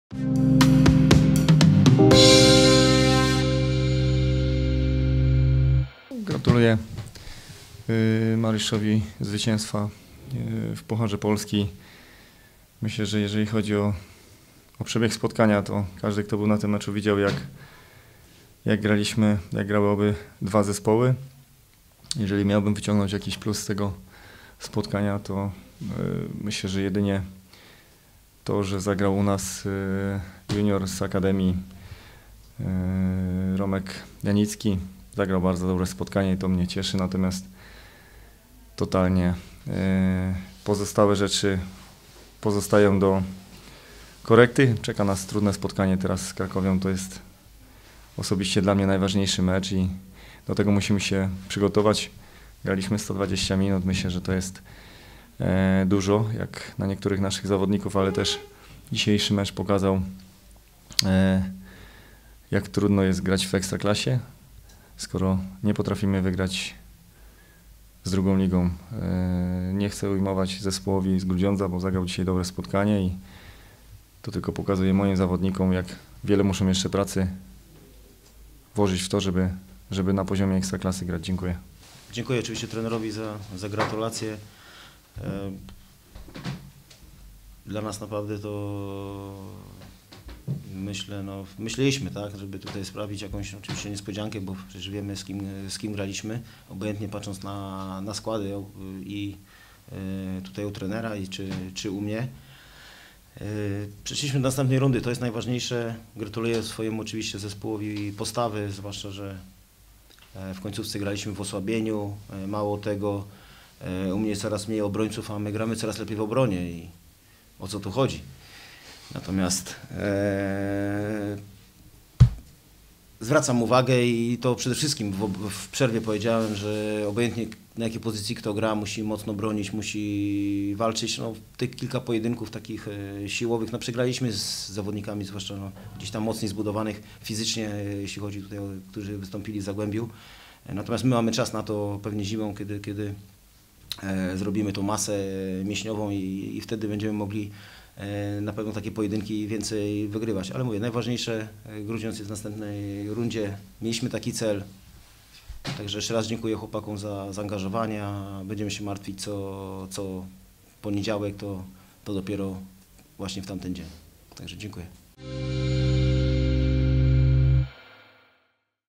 PP: Konferencja po meczu Olimpia Grudziądz (k. 5 - 3) Zagłębie Sosnowiec
Konferencja po meczu 1. rundy Pucharu Polski, w którym Olimpia Grudziądz pokonała Zagłębie Sosnowiec po serii rzutów karnych.